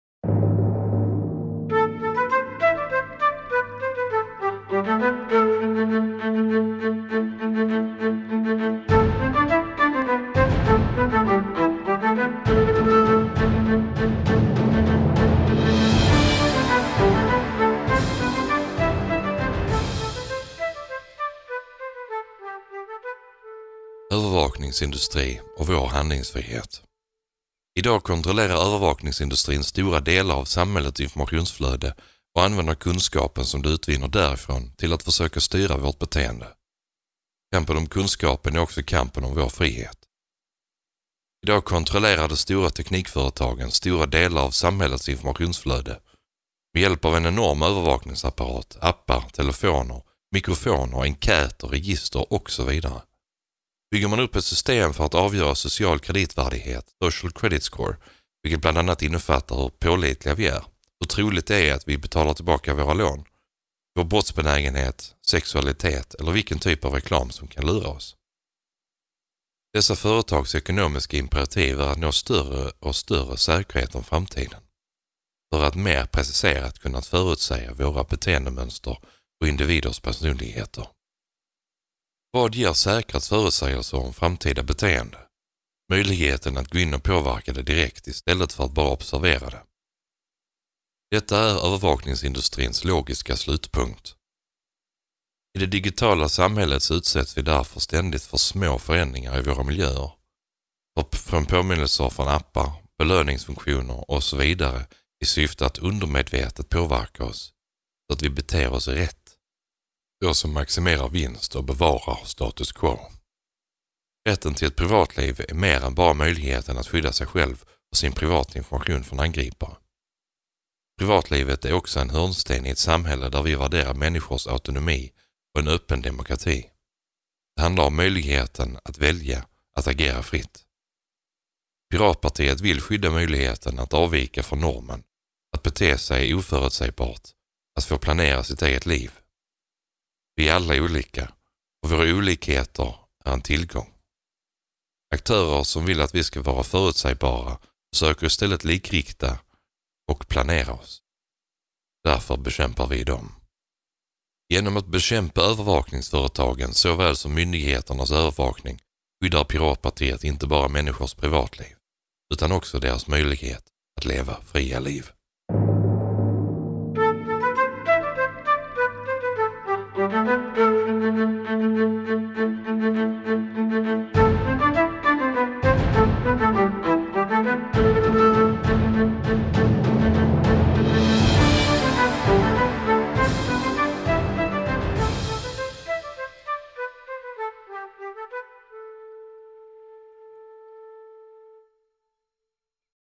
ESKL - Avsnitt 29 - Principprogrammet kapitel 1.2.4
Övervakningsindustrin och vår handlingsfrihet

Högläsning ur Piratpartiets nya principprogram som antogs tidigare i år.

”Idag kontrollerar övervakningsindustrin stora delar av samhällets informationsflöde och använder kunskapen som de utvinner därifrån till att försöka styra vårt beteende. Kampen om kunskapen är också kampen om vår frihet.”